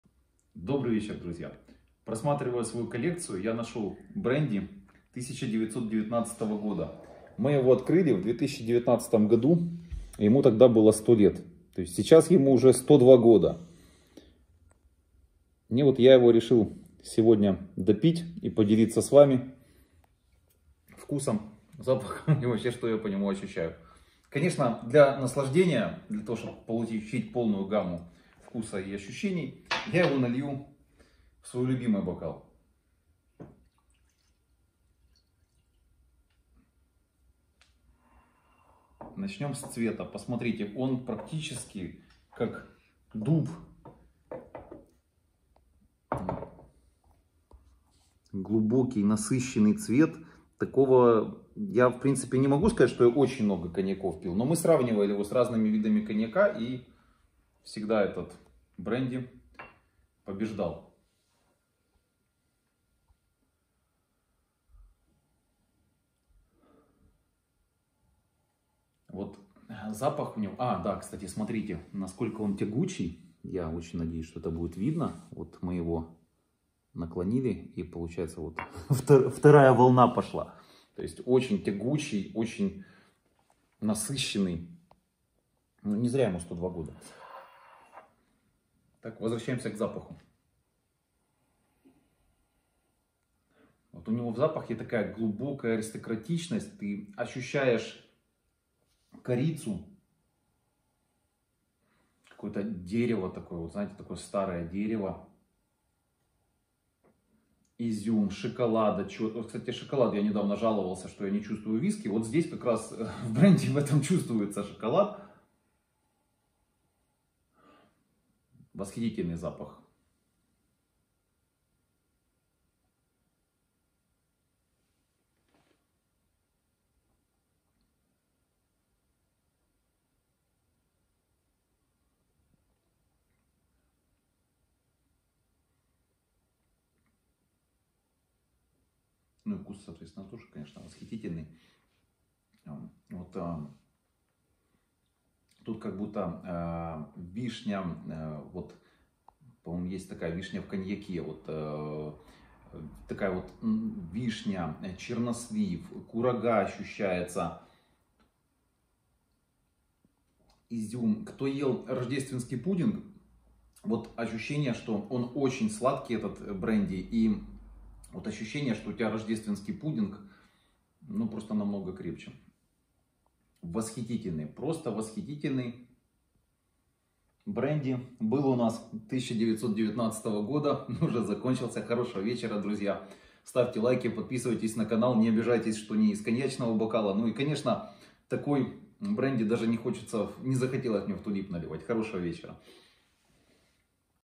102 летний бренди (Hatzipavlou 1919)
На сегодняшний день, это лучший бренди, что пил за всю свою жизнь. Кипрский Бренди Hatzipavlou V.O 1919
Протестируем 102 летний бренди и послушаем отзывы о бокалах, которые 100% напишут :)))
По той истории что мне рассказали.
Абсолютно случайно нашли бренди в бутылках от 1919 в 2000-х годах заказали новые бутылки и разлили его на продажу.
Нигде не сказал, что он 100лет был в бочках.
Хватит писать разную ерунду.